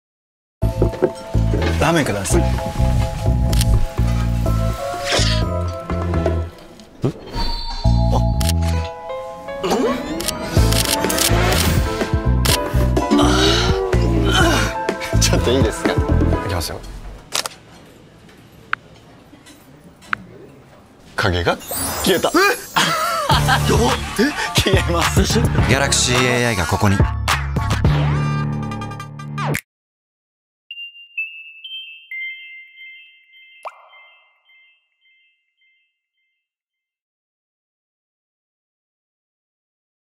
[자막] 나리타 료 "갤럭시 S24 울트라" l NARITA RYO
갤럭시 S24 울트라 광고에 출연한 나리타 료 영상입니다.

Release: Apr. 2024
🔗 0:00 Galaxy S24 Ultra 「成田凌 × 影を消すのもこんなに簡単」

+ 재생목록 더보기!